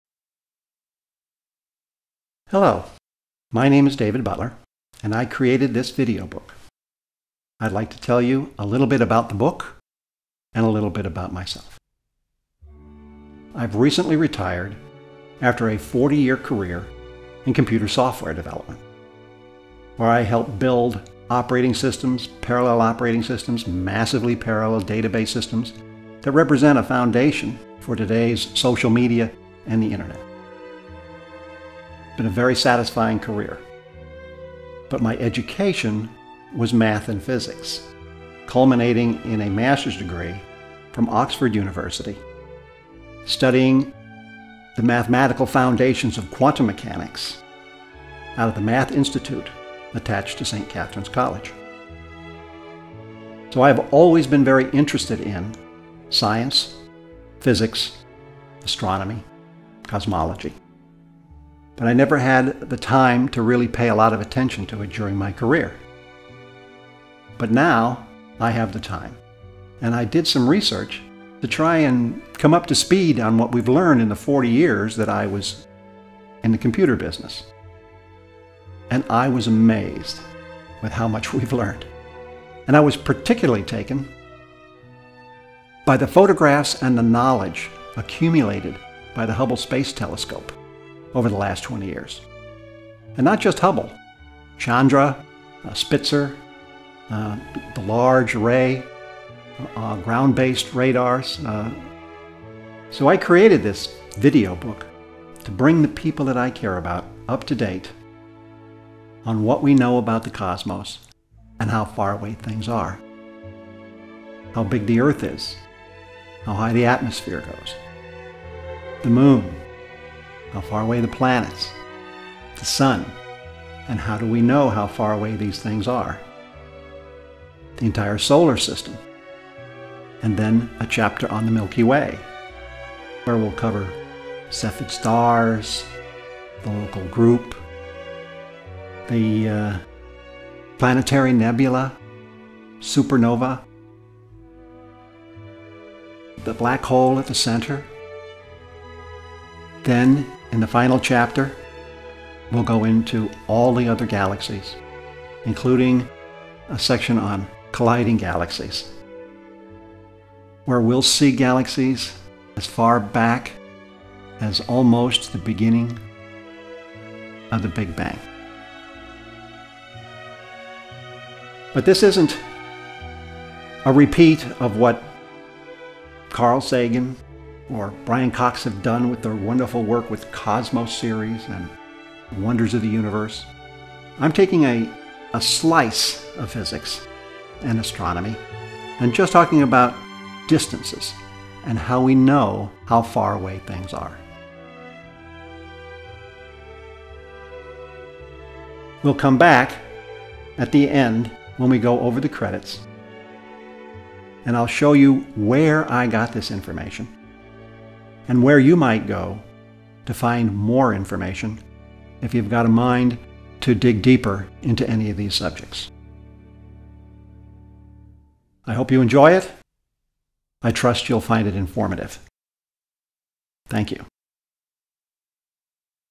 How Far Away Is It - Chapter 1 - Preface
This is a video book of Hubble and other Space Telescope pictures together with an illustrated explanation of how we know how far away things are - from your back yard to the most distant galaxies.

See how:
   triangulation can take us across the globe
   parallax can take us across the solar system
   standard candles can take us across the Milky Way
   red-shift can take us across the universe.

The video book contains three chapters: Solar System; Milky Way; and Galaxies. It also includes a preface and a segment on credits with websites identified that can be used for further research by interested viewers.

Learn about the Sentinels of our Heliosphere. See stars, star clusters, planetary nebulae, supernovae, star forming nebula, black holes, orbiting dwarf galaxies, Andromeda, the local group of galaxies, galaxy superclusters, colliding galaxies and more.

STEM